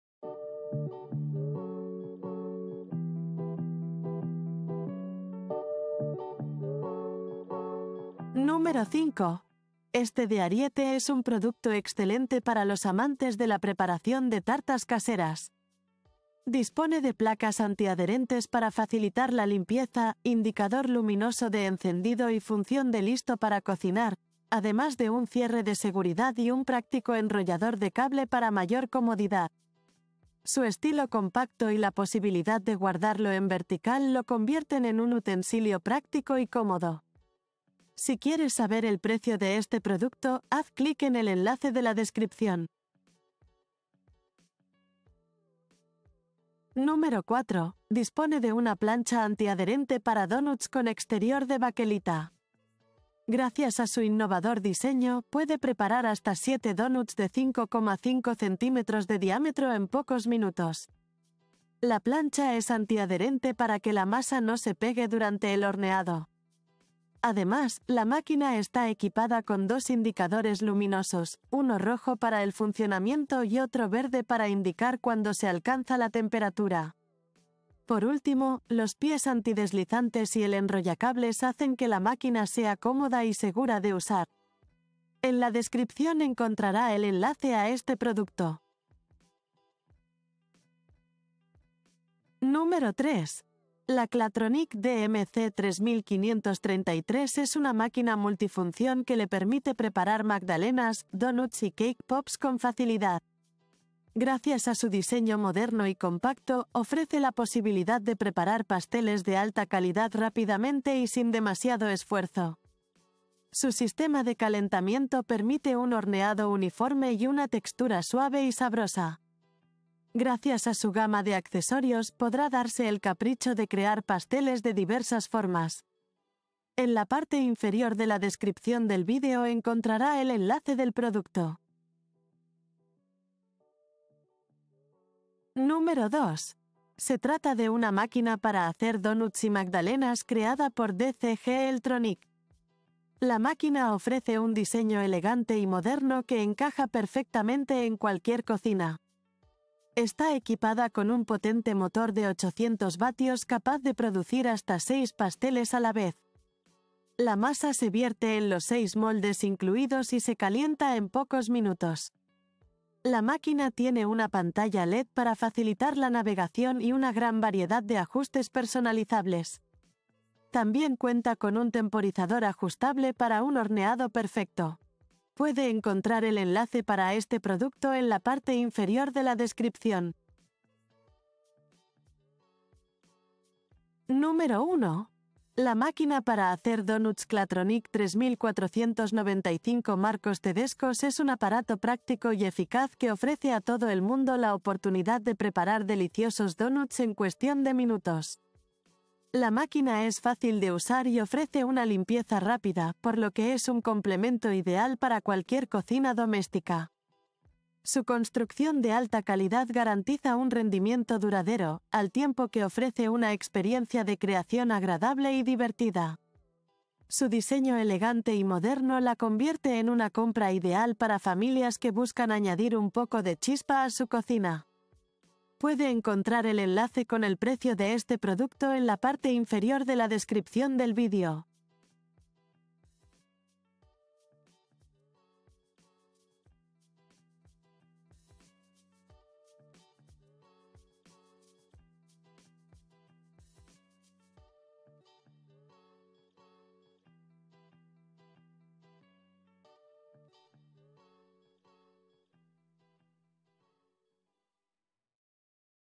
Las 5 mejores máquinas para hacer donuts
1) Clatronic DM 3495 Máquina... » Amazon
2) DCG Eltronic WA2230 6cake... » Amazon
3) Clatronic DMC 3533 - Máqu... » Amazon
4) BEPER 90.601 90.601-Máqui... » Amazon
5) Ariete 189 MÁQUINA DE Don... » Amazon

✋ Aquí están las 5 mejores máquinas para hacer donuts, disponible en el mercado.

00:00 - Intro
00:08 - 5) Ariete 189 MÁQUINA DE Donuts Y Cookies Party Time.
00:59 - 4) BEPER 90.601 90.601-Máquina para Hacer Donuts, 700 W, Rojo.
01:48 - 3) Clatronic DMC 3533 - Máquina de hacer magdalenas, rosquillas, donuts y cake pops...
02:31 - 2) DCG Eltronic WA2230 6cakes 800W Color blanco máquina para madalenas y donuts - M...
03:17 - 1) Clatronic DM 3495 Máquina para Hacer Donuts o Rosquillas, Placa Ant, 900 W, Plás...